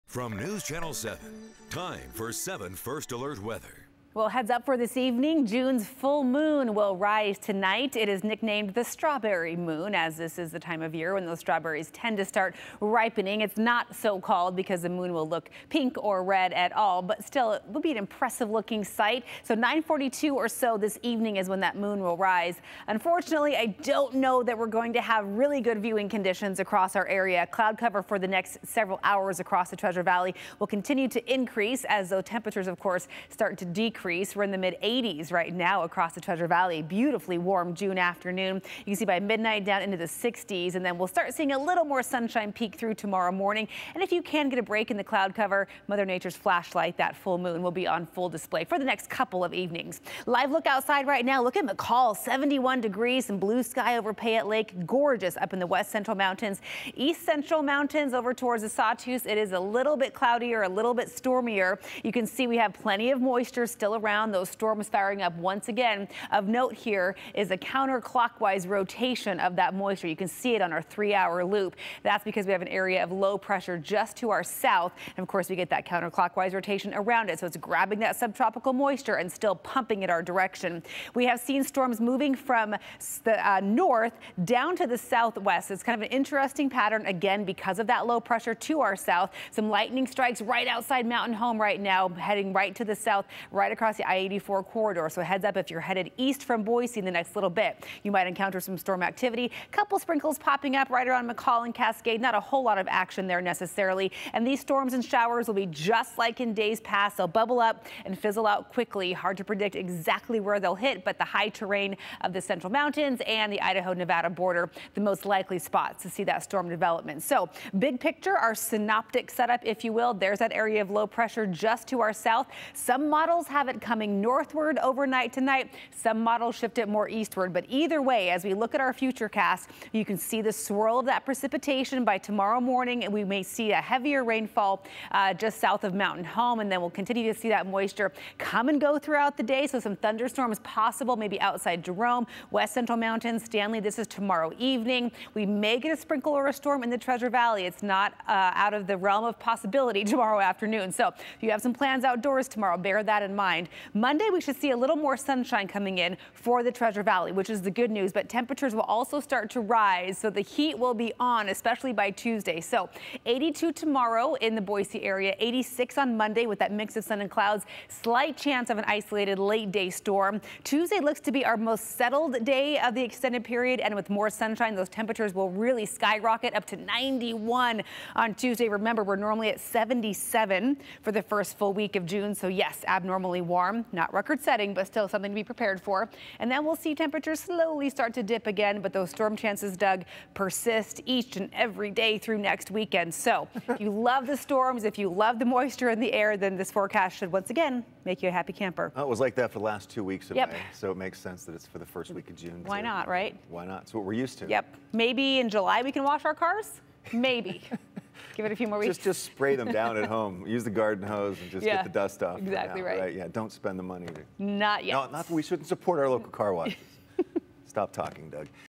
Southern Idaho forecast: Active weather pattern continues for first full week of June
The first full week of June will continue the weather pattern that's been in place for the past several weeks, with warm temperatures and isolated thunderstorms.